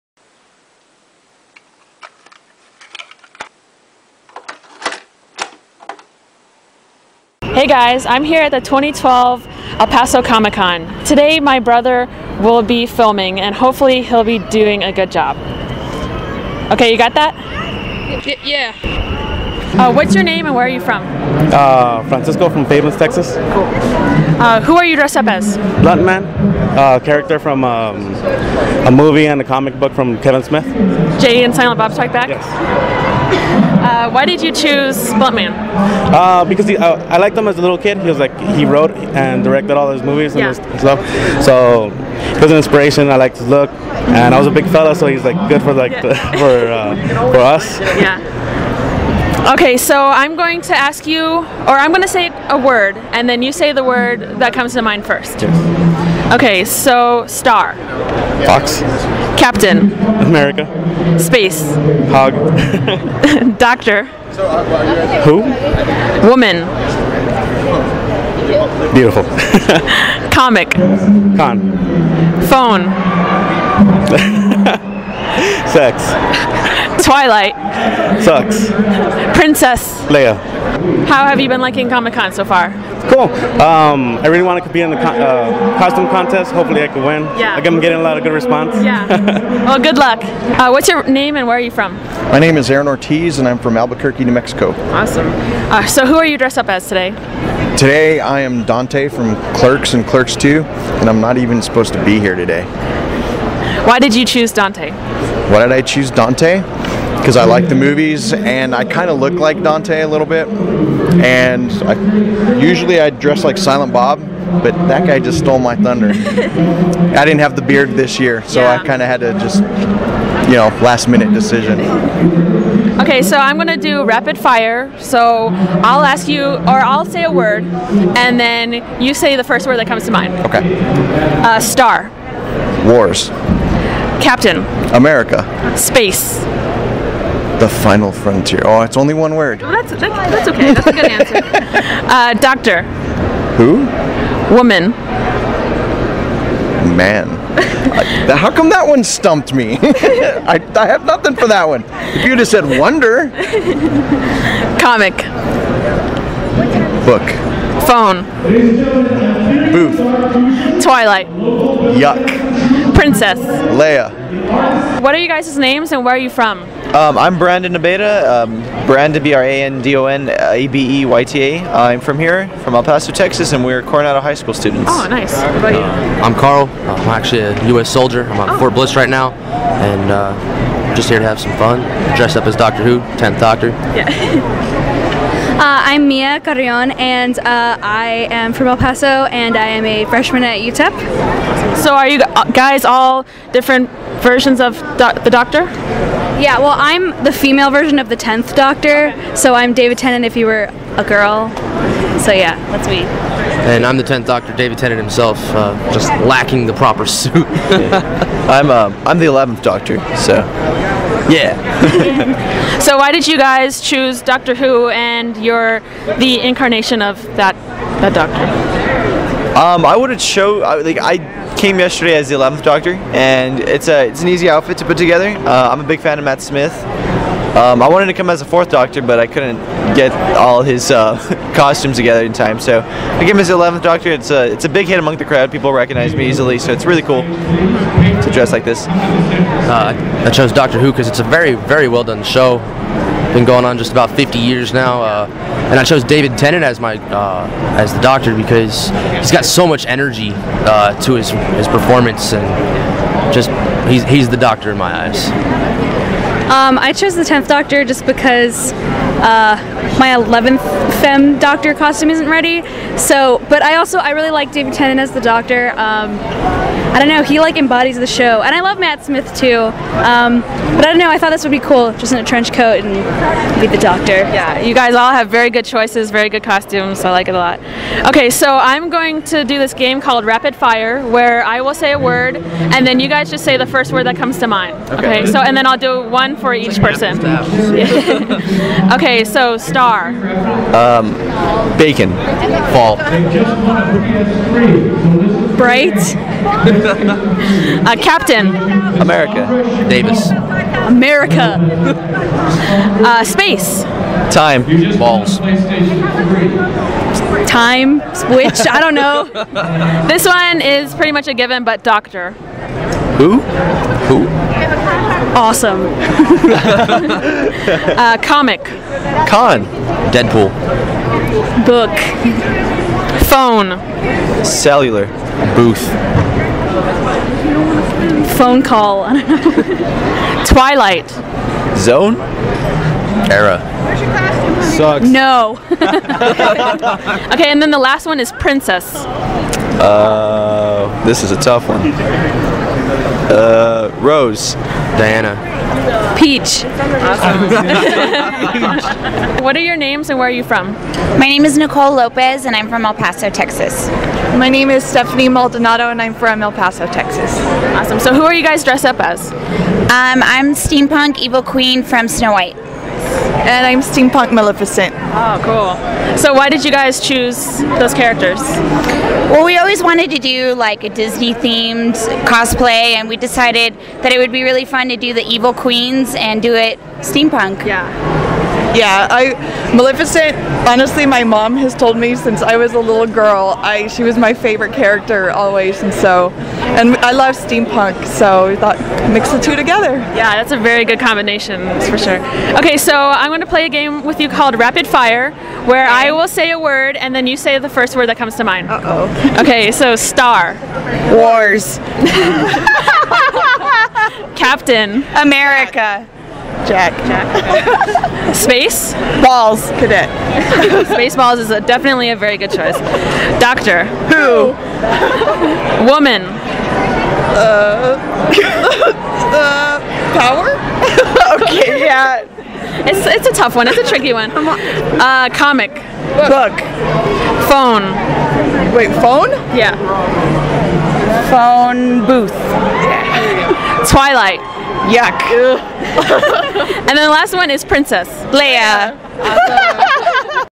El Paso Comic Con 2012 Interviews
I went to EPCON and interviewed people who were cosplaying!